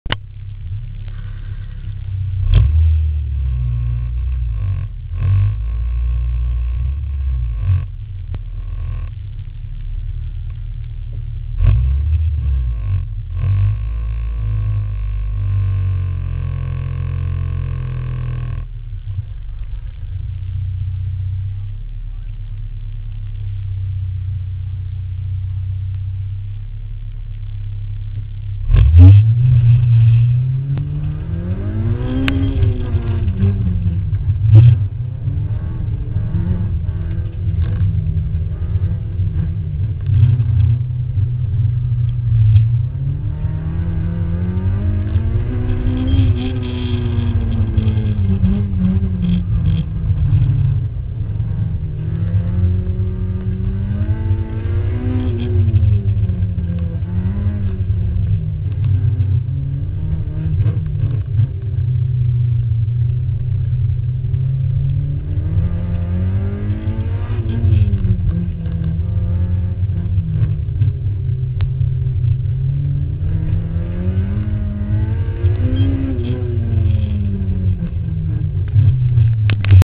VMSC Autocross 04/10/2011 - Paul
K20 EG Civic, Richmond International Raceway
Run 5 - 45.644